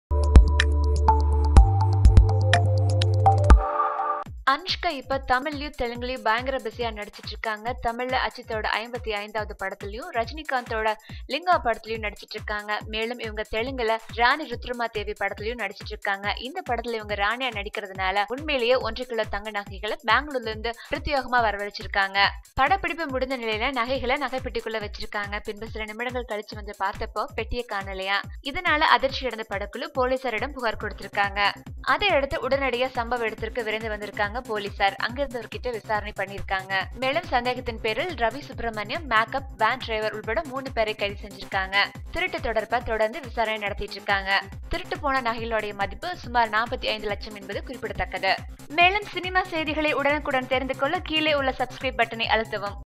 Rs.45 lakhs Gold Jewellery missing in Anushka's Rudrama Devi shooting spot
Anushka's  Real 1.2 Kg Gold Jewellery missing!